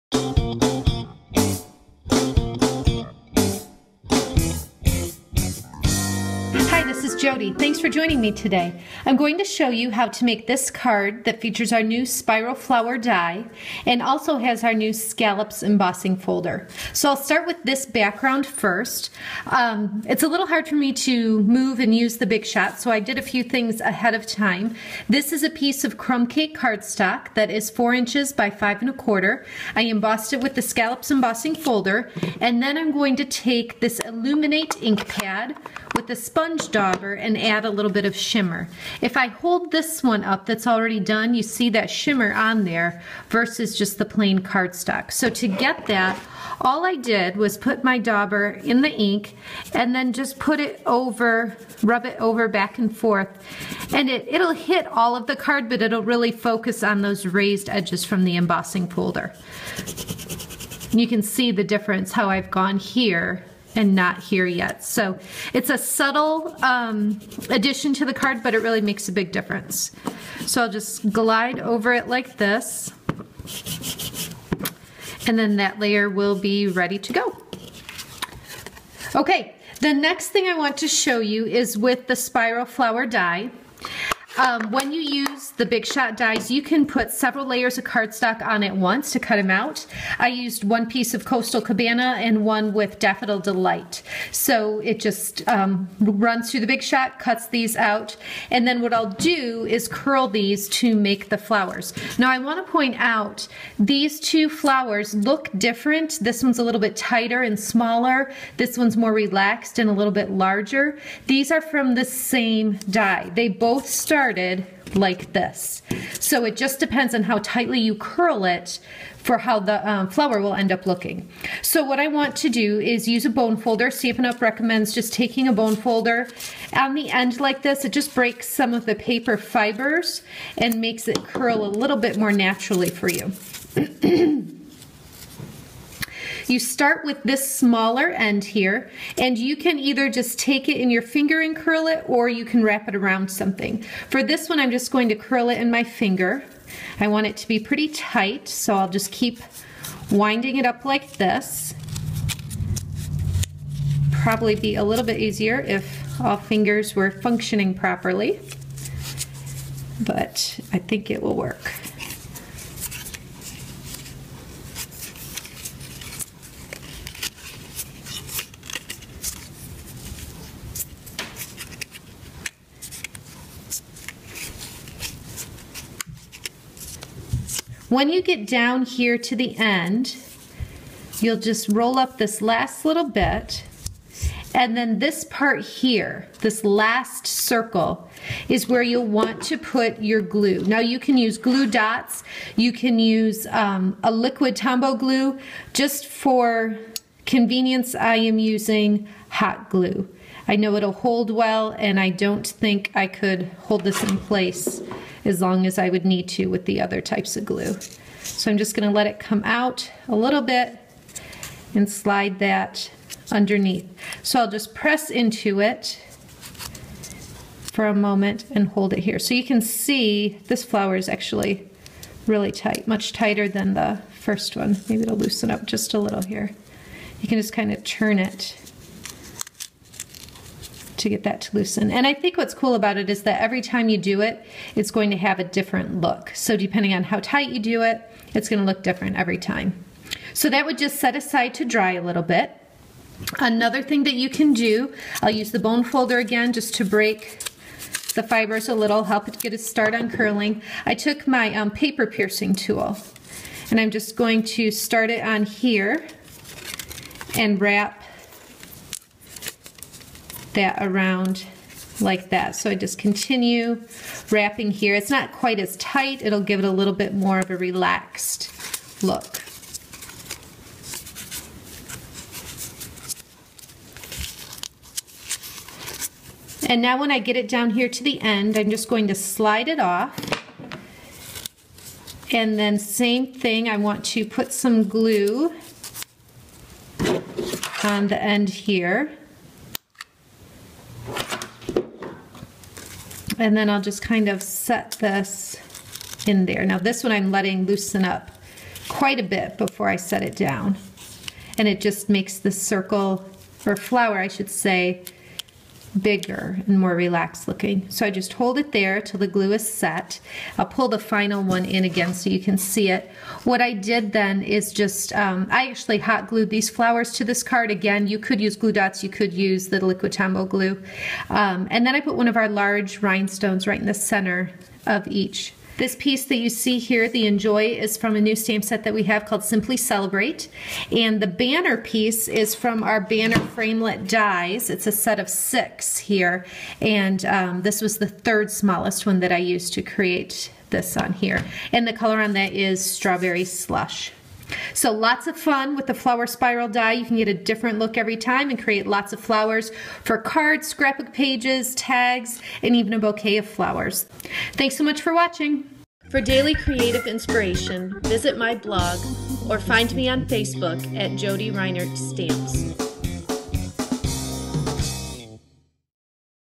Paper Flowers with the Spiral Flower die
Make a variety of flower sizes with the Spiral Flower die for the Big Shot.